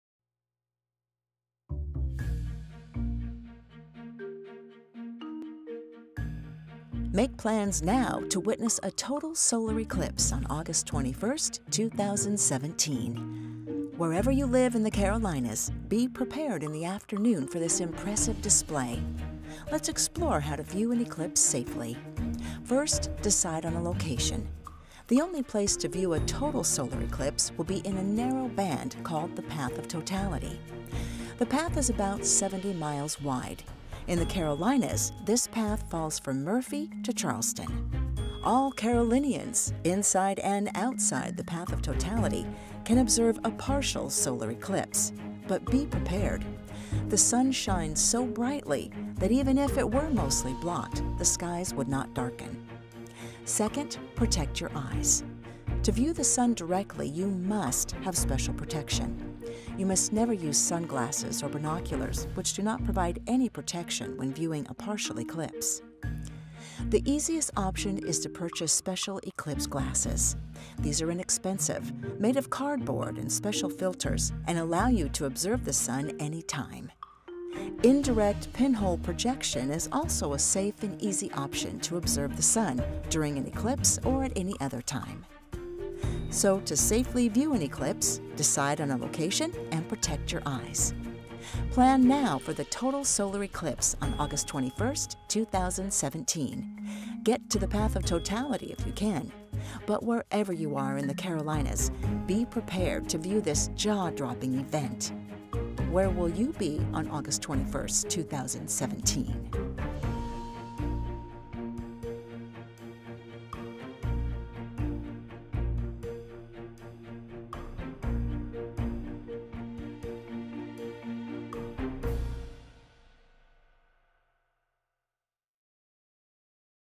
Solar Eclipse - How to View the Solar Eclipse
Make plans now to visit Roper Mountain Science Center on August 21st, 2017 to experience this once-in-a-lifetime event!